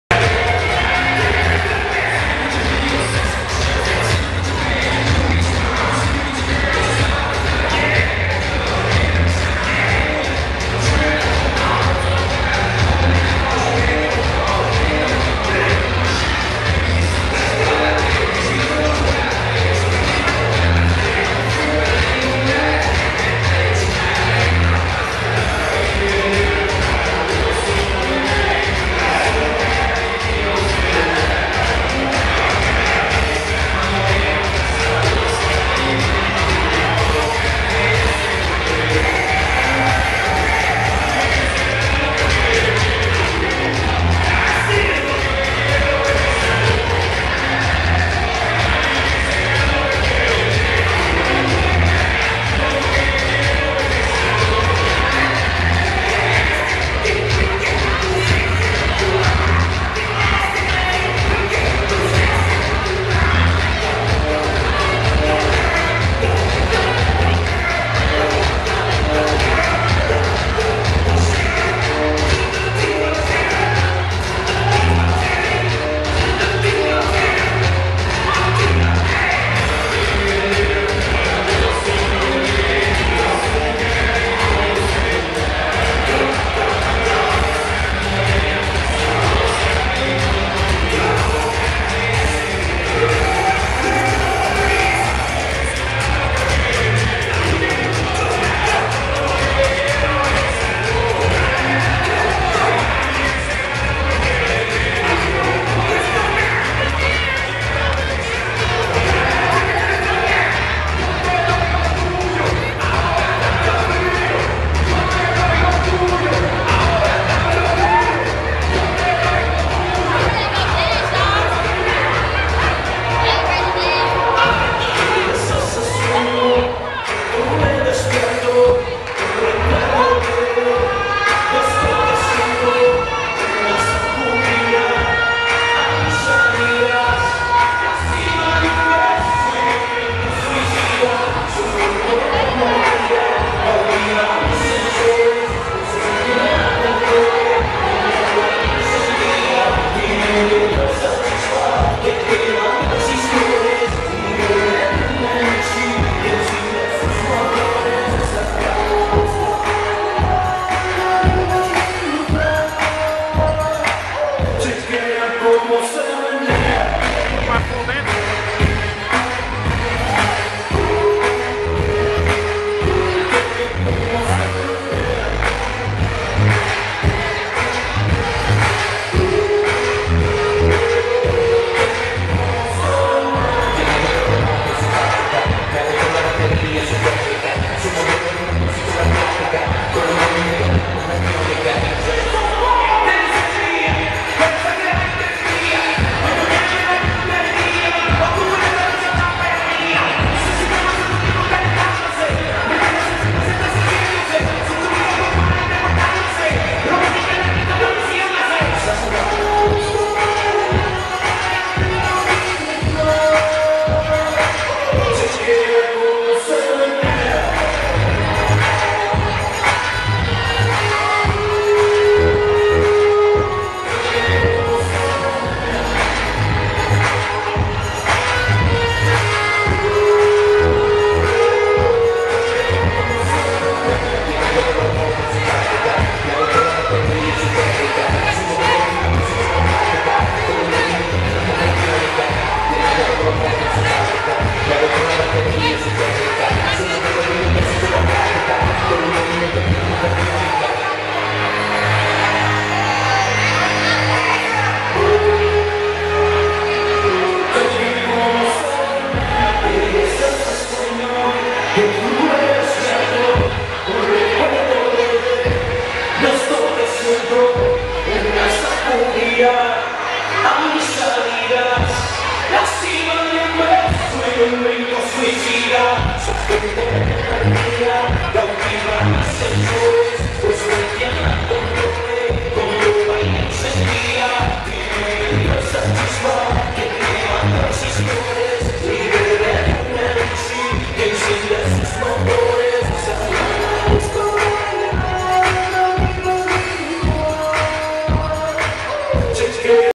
DJ Bally - Osborne High School Prom 2010, GA Intelligent Light Show.MP4
DJ Bally from Exclusive Entertainment Productions provided the High School Prom Light show using 4 Elation Design Spots on 6 ft Global Truss running on Light Factory. The lazer light show was a 200 mW , running sound active. Atlanta, Georgia USA